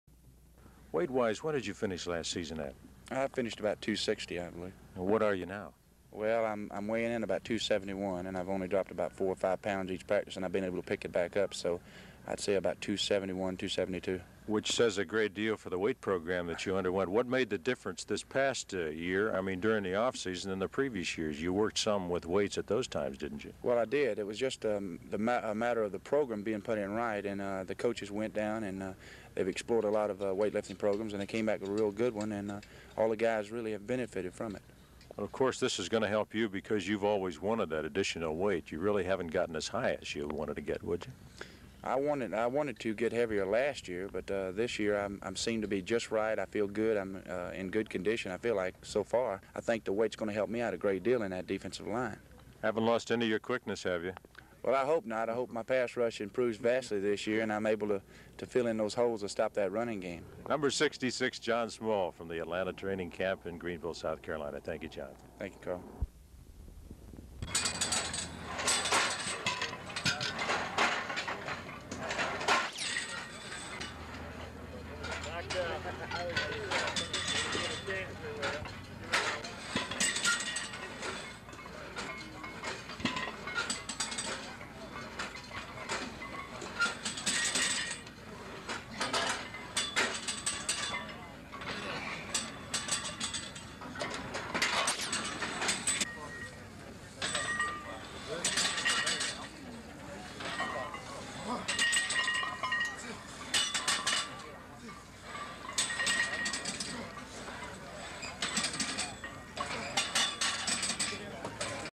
Atlanta Falcons John Small Interview (July 6, 1972)
John Kenneth Small (November 20, 1946 – December 10, 2012 [1]) was an American football Linebacker and Defensive Tackle with the Atlanta Falcons and the Detroit Lions in the National Football League (NFL).[2][3][4]

A native of Lumberton, North Carolina, he played collegiately for The Citadel in Charleston, South Carolina where he was a three-time all Southern Conference selection as well as an Honorable Mention All American pick as a sophomore and junior. In his senior season in 1969 he was a Second-team Associated Press All American and a First-team pick by Sporting News, Time Magazine, and the Newspaper Enterprise Association. He was the First Round pick and 12th overall selection by the Atlanta Falcons in the 1970 NFL Draft; he played for three years with the Falcons and two more with the Detroit Lions, retiring after the 1974 season.

Small was a recipient of the Order of the Palmetto, the highest award given by the state of South Carolina; in 1977 he was a member of the inaugural class of the Citadel Athletic Hall of Fame. In 1988 he was inducted into the South Carolina Athletic Hall of Fame and that same year was named to the Southern Conference 75th Anniversary team.[5][6]

After football, Small was a speaker at schools and hospitals in the Augusta area. He helped create a Christian ministry program to help troubled youth called JKS Team Incorporated (where he led as CEO).[7]